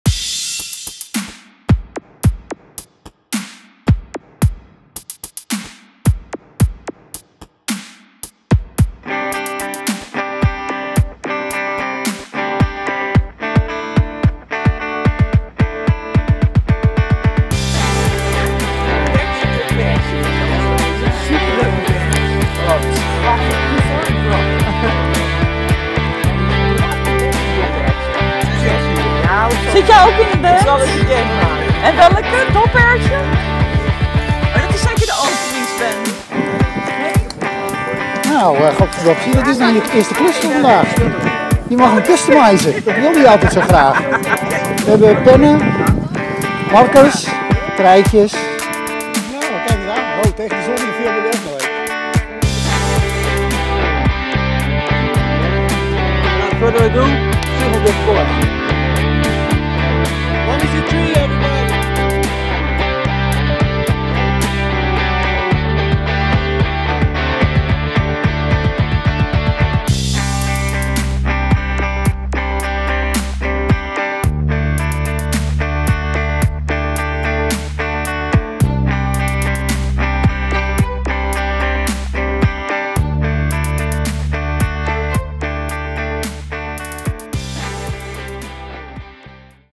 LiveXS Reünie Comeback Picknick 2019 (Aftermovie)
Een korte samenvatting van de LiveXS Reünie Comeback Picknick 2019 met akoestische optredens van Civil Discourse, Mummy's a Tree, Sean Gascoigne, Hein Augustijn & Brabomundo, Before the Drop, Christopher Max, The Pheromones, Charmless i en Bertje Doperwtje. Kijk deze week naar deze playlist op YouTube voor alle optredens en interviews.